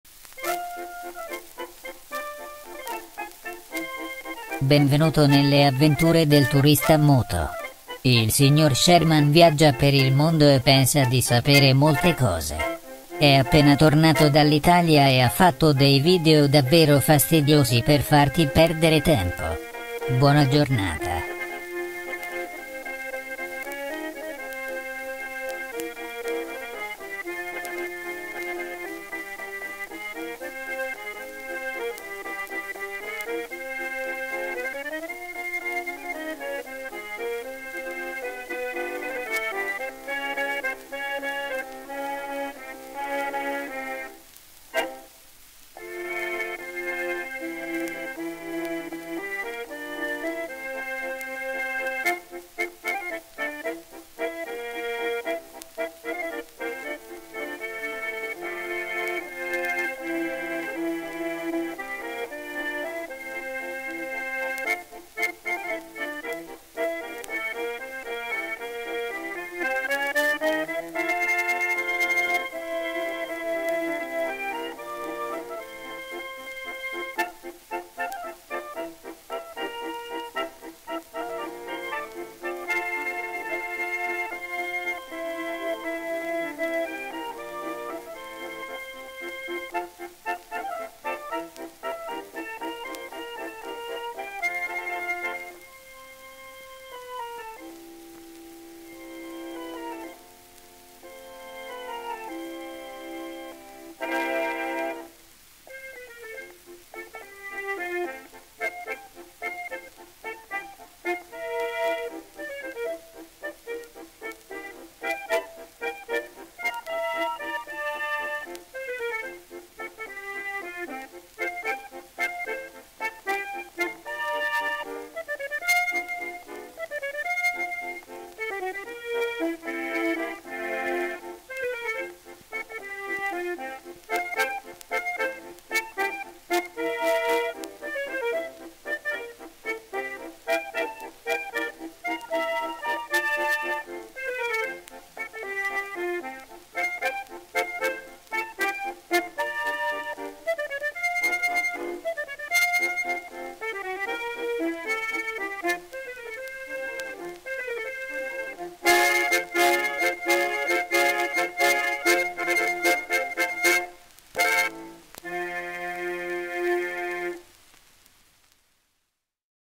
The Dumb Tourist in Italy
Mr. Sherman went to Italy! Italy is a great place to visit, especially if you're a dumb tourist like him. Enjoy a preview of the time-wasting videos from his trip, especially the bad jokes and other dopey things that put the "ANNOYING" in MR. SHERMAN'S ANNOYING VIDEOS.